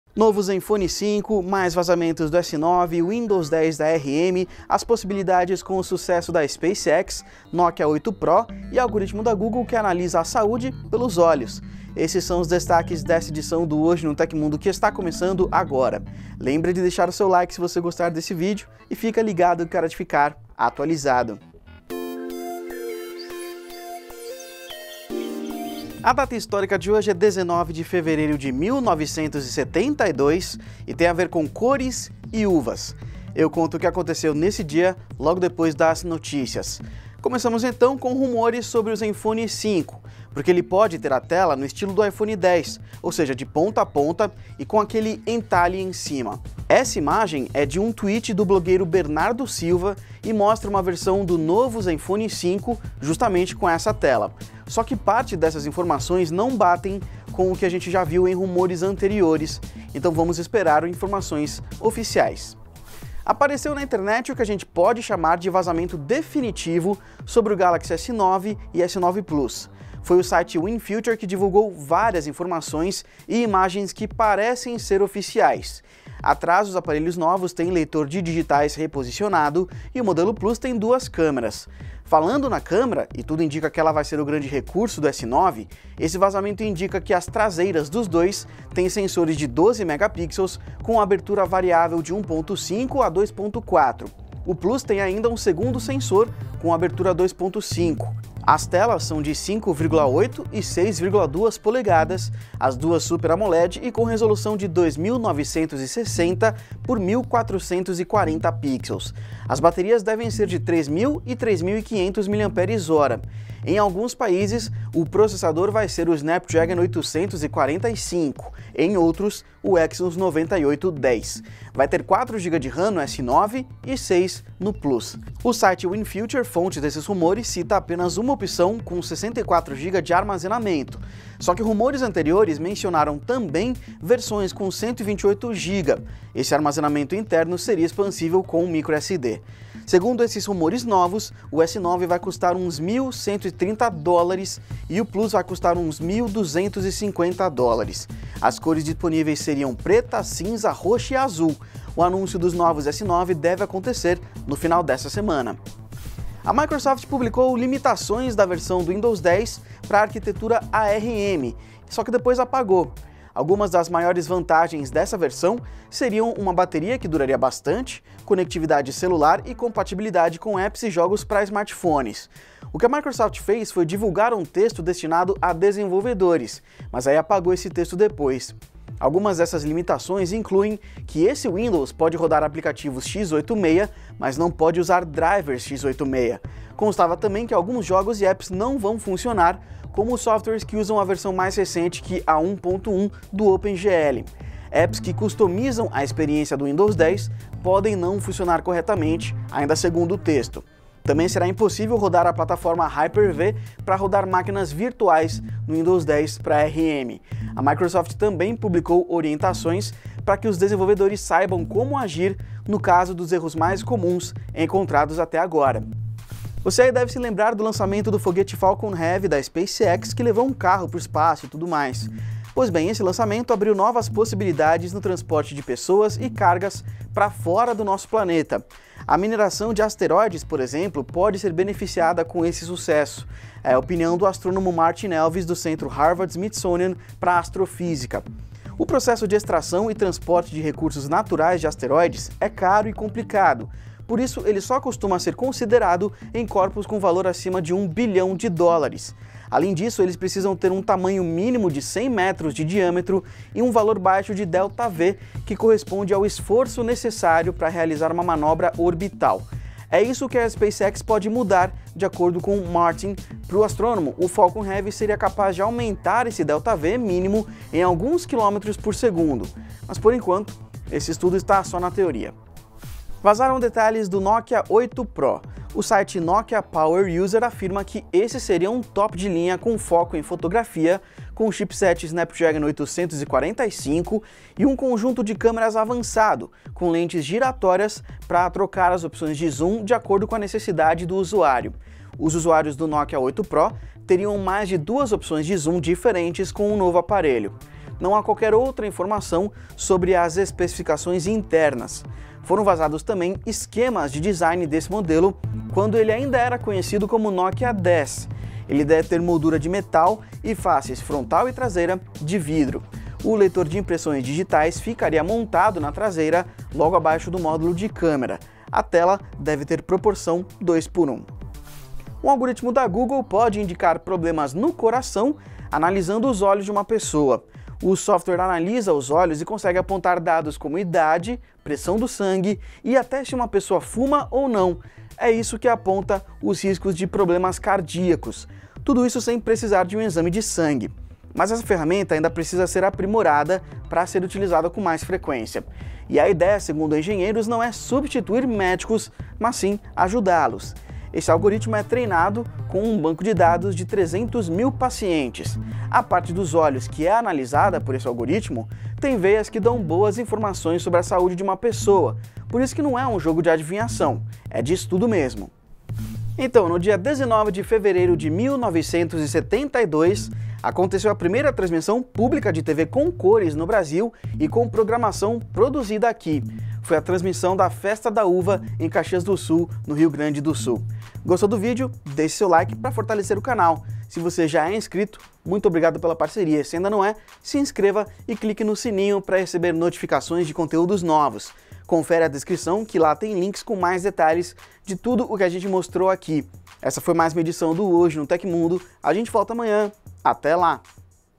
Novo ZenFone 5, Samsung Galaxy S9, Windows 10 ARM, SpaceX, Nokia 8 Pro e mais - Hoje no TecMundo
:: Hoje no TecMundo 19/02 ::

00:37 ZenFone 5 também vai aderir à moda da tela no estilo do iPhone X [rumor]

01:03 Vazamento gigantesco mostra tudo sobre os novos Galaxy S9 e Galaxy S9+

02:36 Microsoft publica limitações do Windows 10 para ARM... e depois apaga

03:36 Novo foguete da SpaceX pode revolucionar a mineração de asteroides

04:33 Vazam detalhes do “Nokia 8 Pro”: monstro fotográfico com lentes giratórias

05:22 IA da Google pode prevenir doenças cardíacas apenas examinando seus olhos

CRÉDITOS
Texto: Danilo Amoroso
Apresentação: Danilo Amoroso
Imagens: Danilo Amoroso
Edição de vídeo: Danilo Amoroso
Motion Graphics: Gustavo Silva
Arte da thumb: Silmara Slobodzian

Temos de comparador de celulares até plugin para Chrome que facilita sua vida na busca de uma oferta especial.